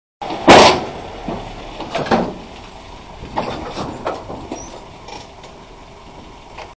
Juweeltje op weg naar werkplaats in Leiden
Vandaag gaat in de boeken als een nostalgische dag voor Motorhuis Leiden. De uit 1975 afkomstige Opel Rekord 1700, welke sinds 20 jaar niet meer op de weg te vinden was, is sinds vandaag, mede door Motorhuis Leiden weer on the road!